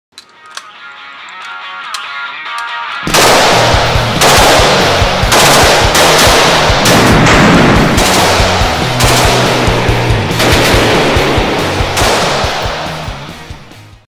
Gun Range
I am working on a small documentary showcasing security teams!  Hope you enjoy the promo and the full film will be out by the end of March!